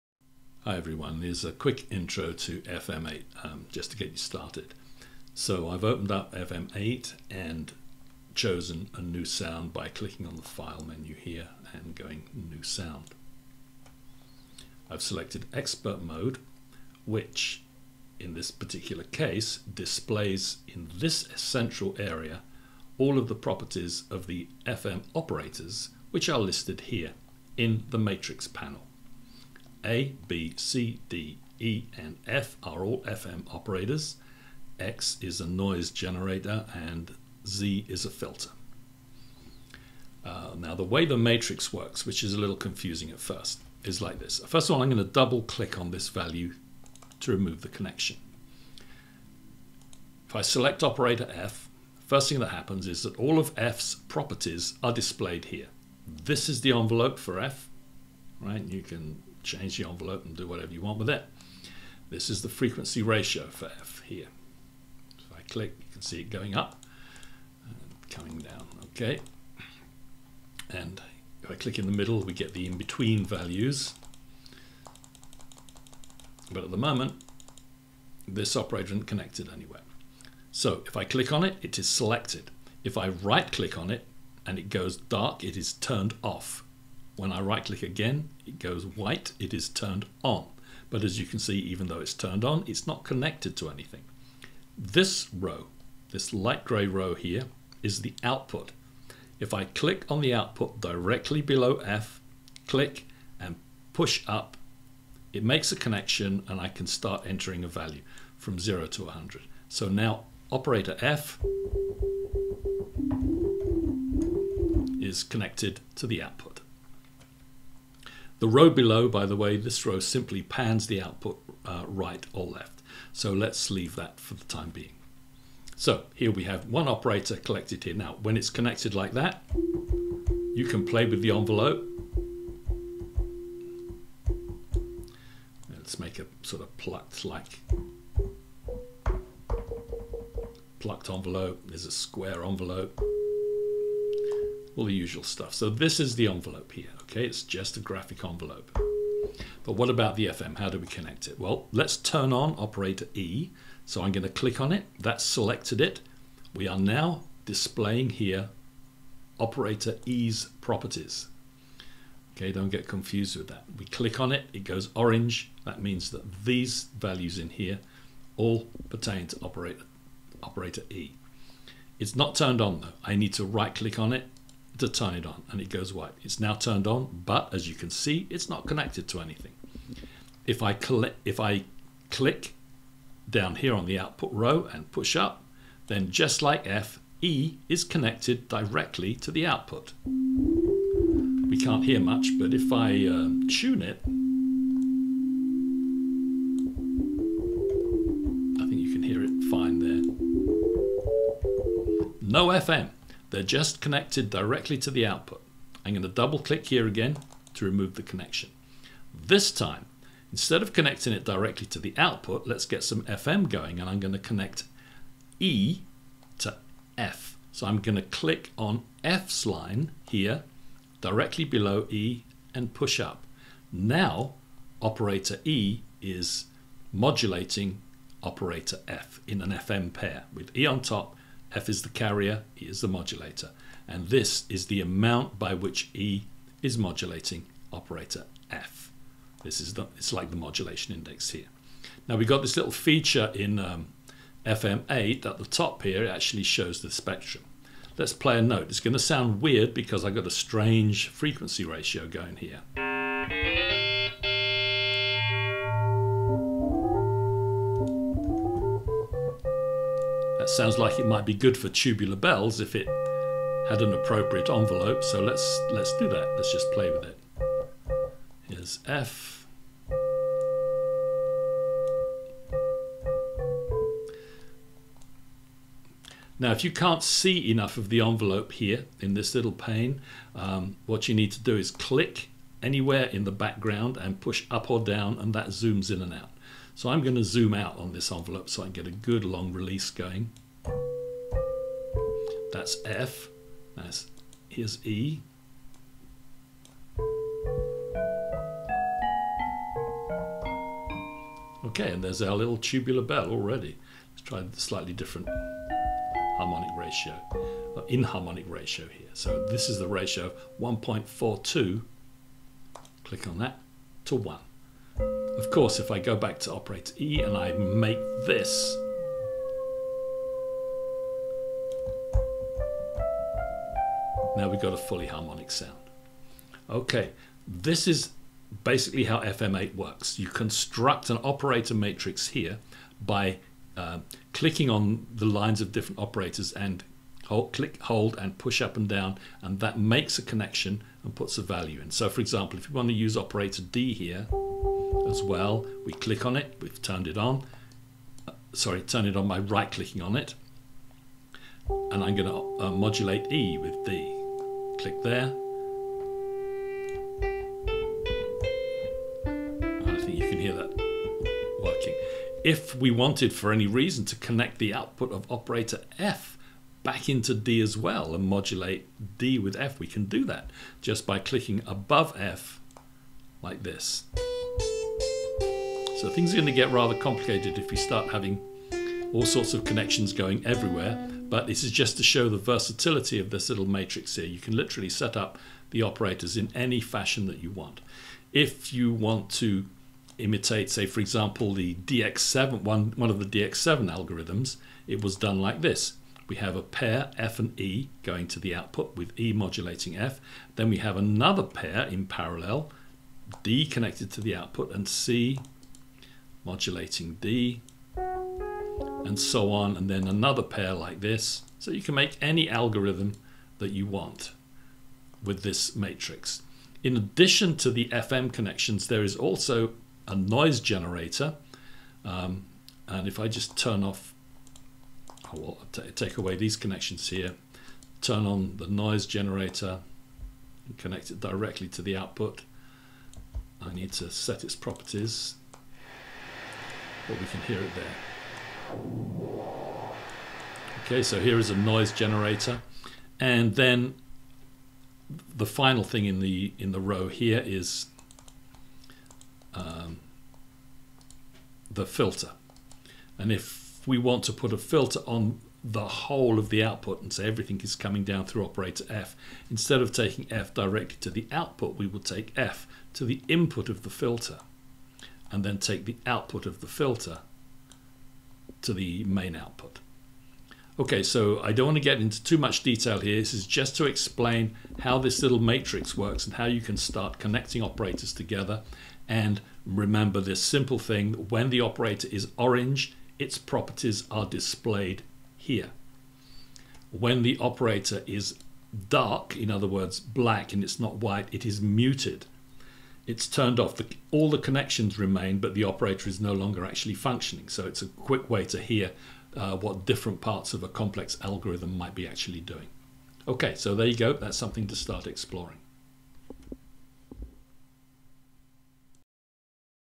Basic Introduction to FM8 - part 1
Very quick intro to FM8 and it's operator matrix from Native Instruments. Does not include info on FM theory.